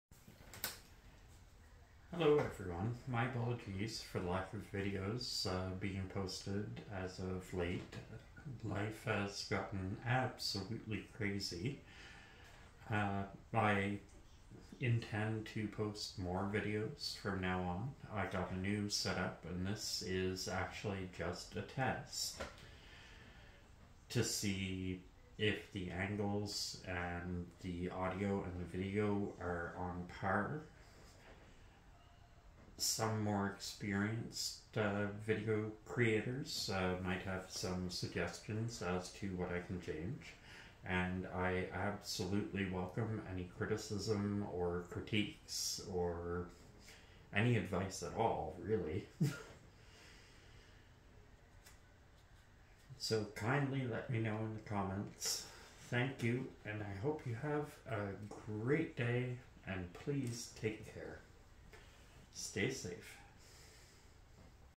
new camera setup, need advice
i finally have a proper tripod mount so i can start making new videos.  i would like any advice from more experienced people in this space.  I'm honestly a newb when it comes to this sort of thing.  so any suggestions are appreciated.  thank you in advance.